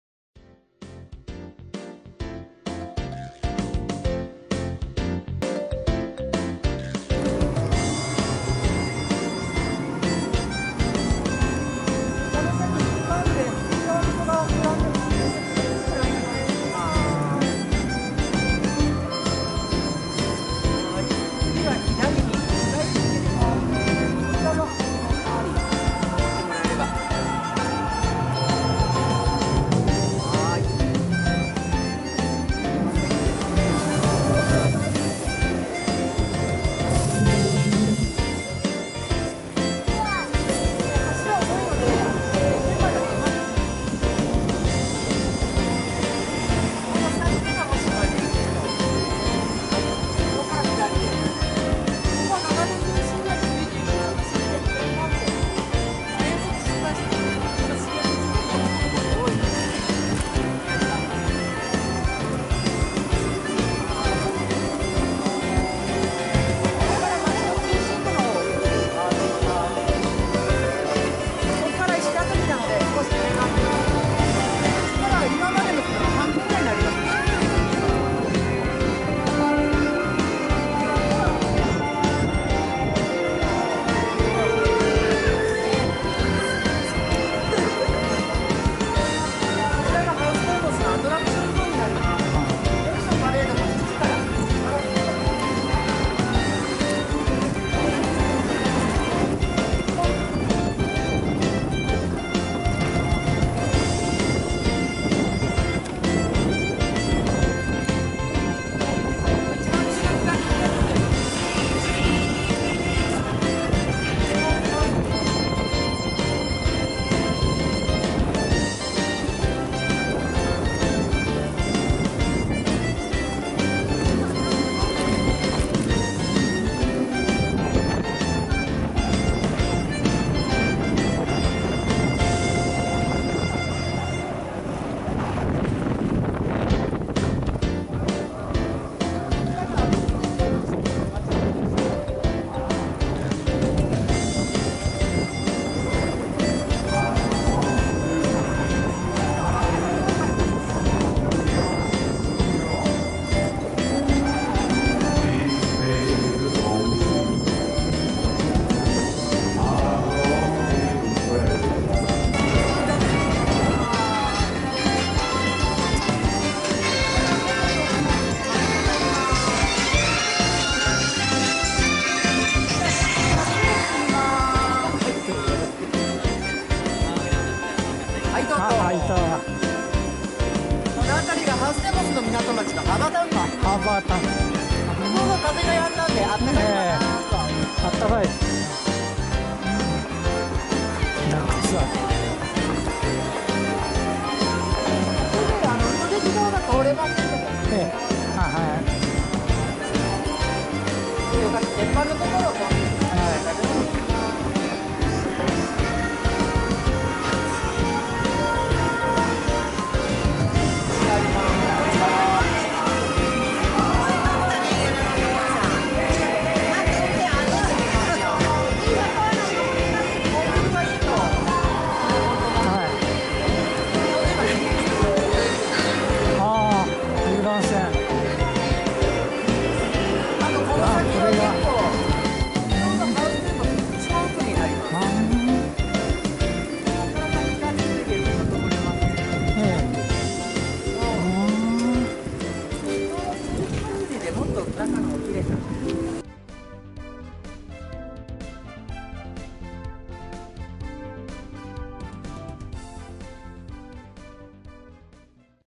ハウステンボスで遊ぶ 2015 セグウェイで周遊する
ハウステンボスHuis Ten Boschは長崎県佐世保市にあり、オランダの街並みを再現したテーマパークであり、オランダ語で森の家を意味する言葉です。今回、広大な場内をセグウェイを使い、50分かけ、散歩することができました。「セグウェイ」とはアメリカで発明された電動立ち乗り二輪車です。アクセルやブレーキ操作なしに重心移動によって移動できます。試乗前に簡単なトレーニングをすれば、だれでも簡単に乗ることができます。日本では原則として、公道を走ることができませんが、いろいろな場所で試乗体験することができます。